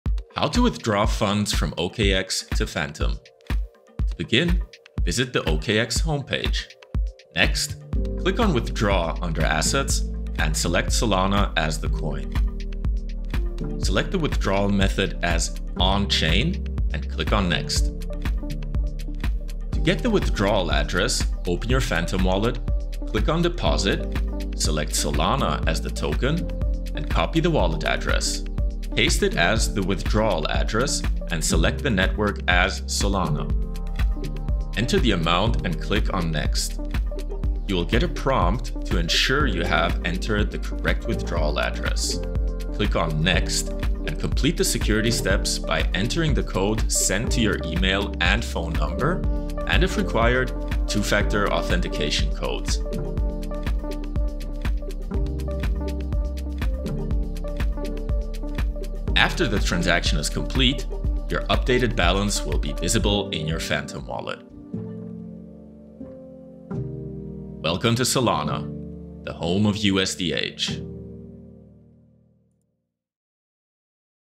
How to Send SOL from OKX to Phantom Wallet [Step-by-Step Tutorial] | Solana Phantom Wallet Extension
This video will show you how to send SOL from OKX to Phantom. Learn how to send SOL to Phantom and participate in DeFi on Solana in minutes

This tutorial will show you how to withdraw SOL from OKX, a popular cryptocurrency exchange, to your Phantom wallet. $SOL is the native token of the Solana blockchain, and Phantom is a popular wallet that supports SOL and other tokens on Solana.

This tutorial will describe:

-How to access your OKX account and navigate to the withdrawal page
-How to create a new SOL withdrawal request and fill in the necessary details
-How to verify your withdrawal request and submit it for processing
-How to confirm your withdrawal status and receive your SOL in your Phantom wallet